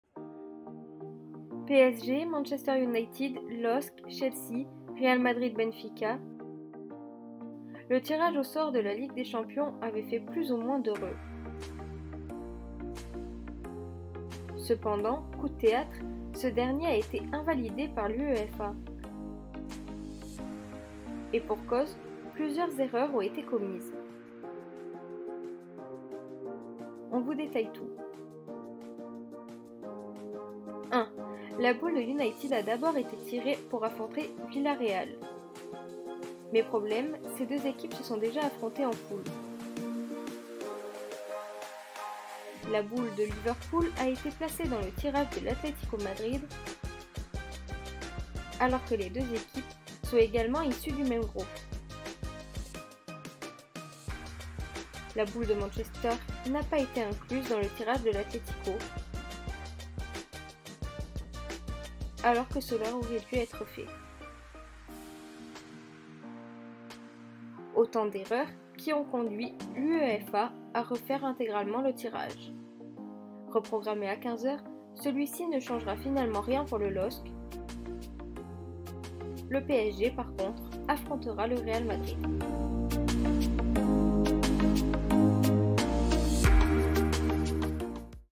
PSG, Manchester United, LOSC, Chelsea, Real Madrid… mais pourquoi le tirage de la LDC été invalide ?
« Suite à un problème technique avec le logiciel d'un prestataire de services externe qui indique aux officiels quelles équipes sont éligibles pour s'affronter, une erreur matérielle s'est produite lors du tirage au sort des huitièmes de finale de l'UEFA Champions League. » Voilà comment a réagi l’UEFA suite au tirage invalide de ce lundi. Mais s’il a été refait, dans de meilleures conditions, savez-vous pourquoi il était invalide ? On vous explique tout !

La musique vous est proposée par « La musique libre ».

Par Vanessa Maurel et Ruben Dias.